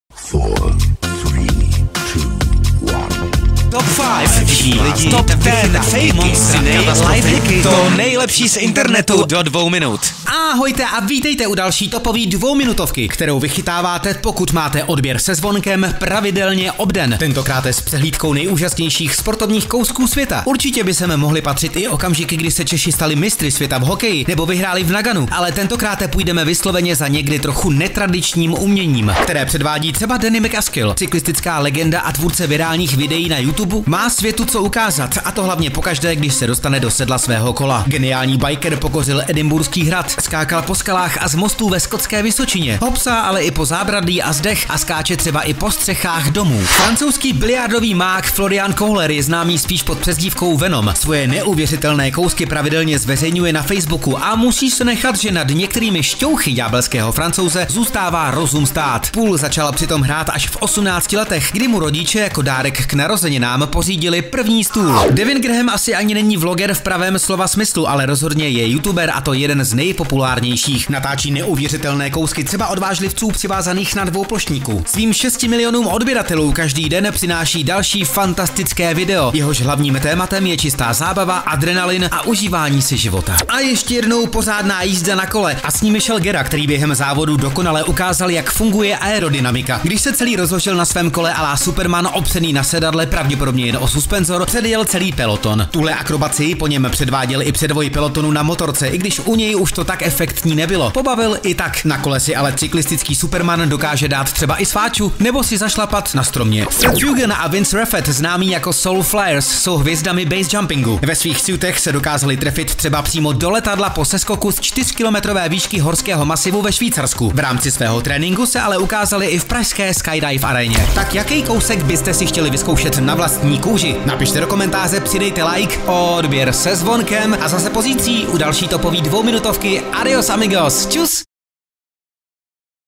TOP 5 nejúžasnějších SPORTOVNÍCH VÝKONŮ
S tímhle talentem a odvahou se prostě člověk musí narodit. Nakonec přivázat se k dvouplošníku a vyzkoušet si let na jeho křídlech, nebo skočit ze skály ve speciálním obleku, se kterým přistanete za letu do letadla, nedokáže každý. Takové sportovní šílenosti provádí například i Michael Guerra, ovšem pro změnu v plné rychlosti na kole. Zatímco diváci z toho mají husí kůži, tito sportovní borci si to prostě užívají. Vy teď budete moct vyzkoušet obojí. Budete si užívat i tu husí kůži. Tohle se totiž hned tak nevidí. TOP 5 nejúžasnějších SPORTOVNÍCH VÝKONŮ vám posíláme ... a to jak jinak, než do 2 minut.

Staň se členem do 2 minut Amigo❤️
Získej výhody a bonusy:
PŘIPOJIT SE